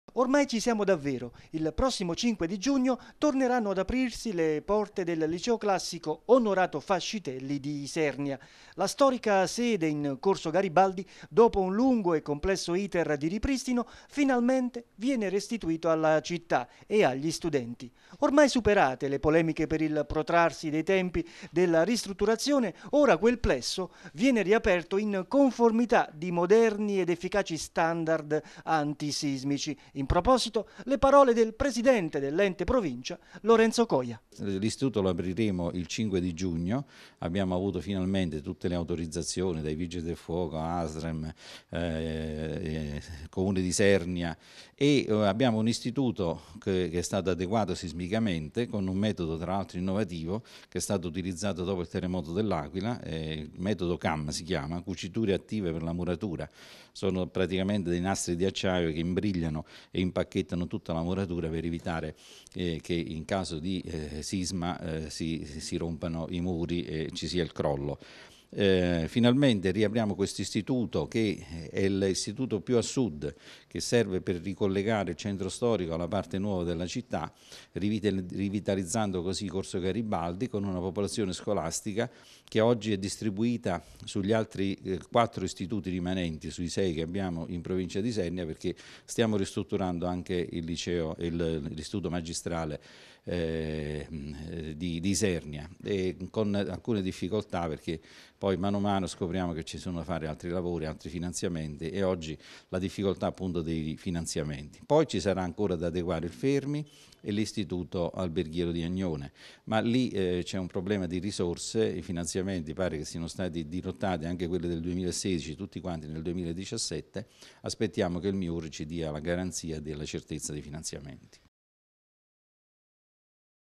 Liceo "Fascitelli"
Ormai ci siamo davvero. Il prossimo cinque di giugno  torneranno ad aprirsi le porte liceo classico ‘Onorato Fascitelli’ di Isernia. La storica sede in corso Garibaldi, dopo un lungo e complesso iter di ripristino, finalmente viene restituito alla città a agli studenti. Ormai superate le polemiche per il protrarsi dei tempi della ristrutturazione, ora quel plesso viene riaperto in conformità di moderni ed efficaci standard anti sismici. In proposito le parole del presidente dell’ente provincia Lorenzo Coia